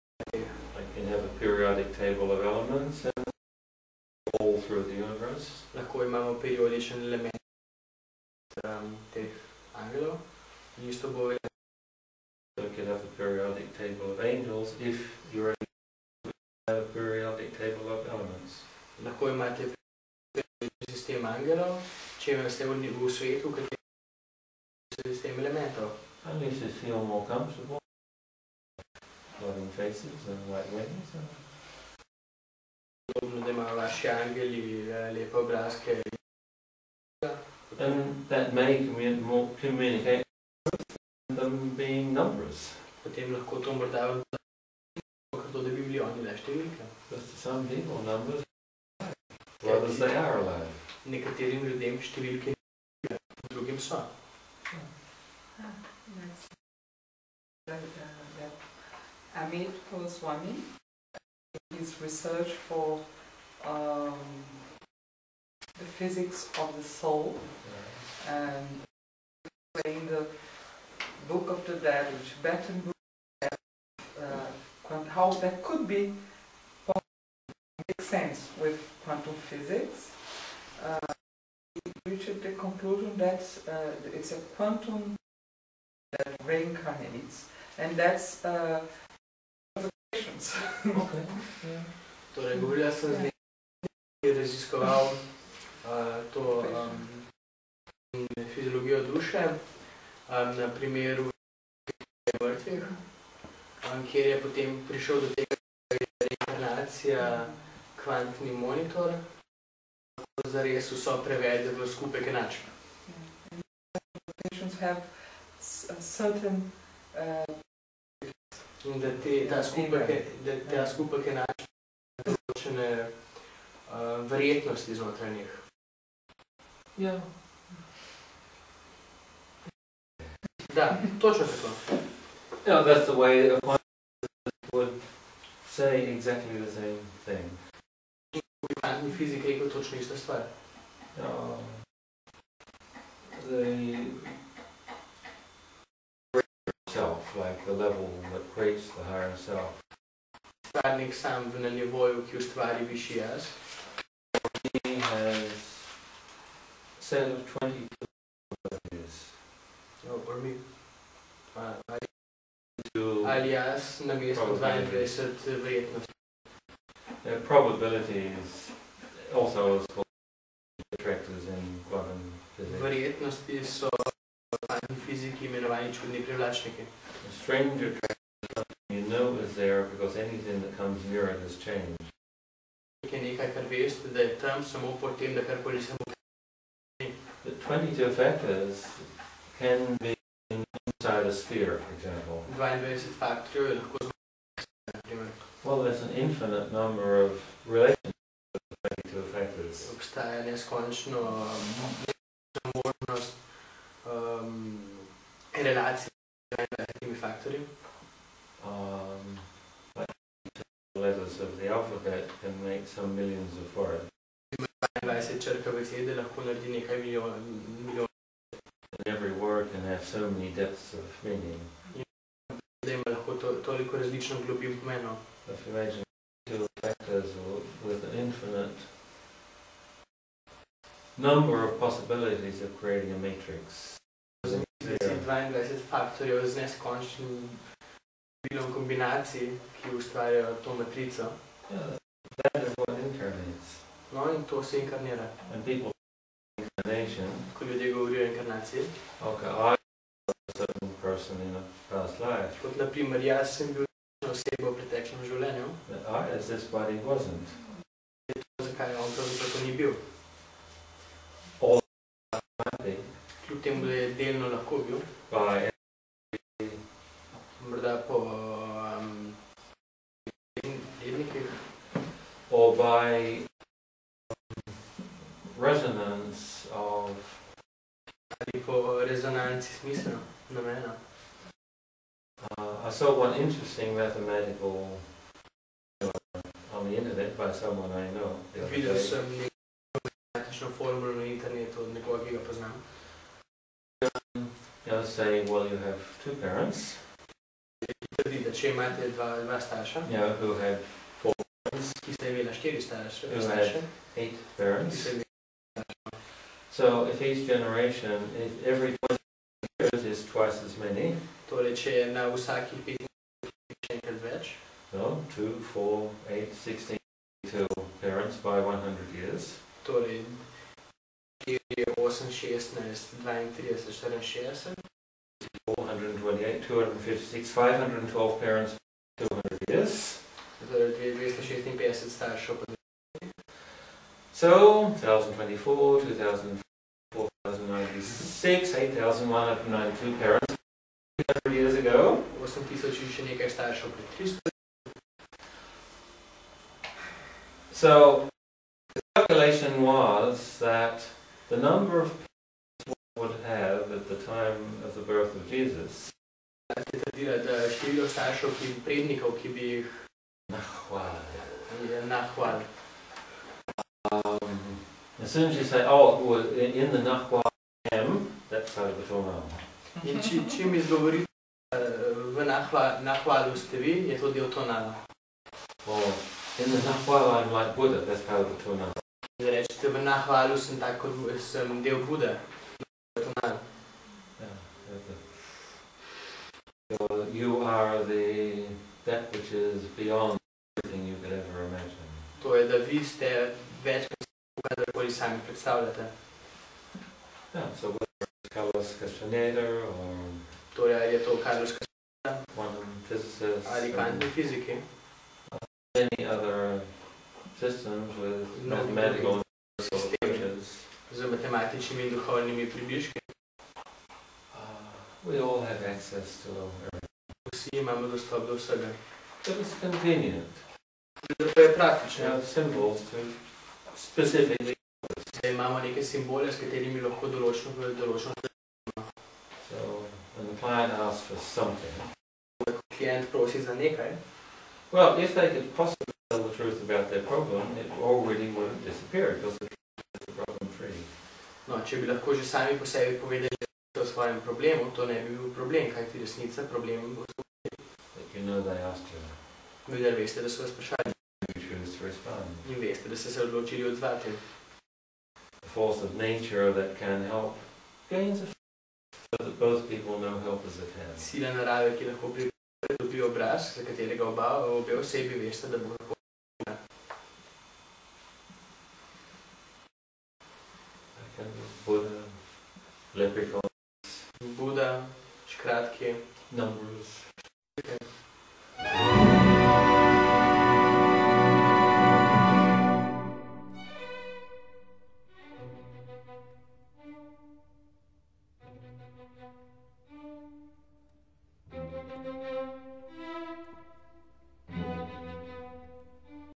Periodic Table of Angels and Plants 3PART OF3
Over the course of 50 years, Rowland has gathered enough information to fill 17 books and wish to assemble them as his legacy for this and future generations. His system is proven to succeed and has been used the world over for curing drug addiction, poverty, and lifting people out of warfare and chaos mentalities.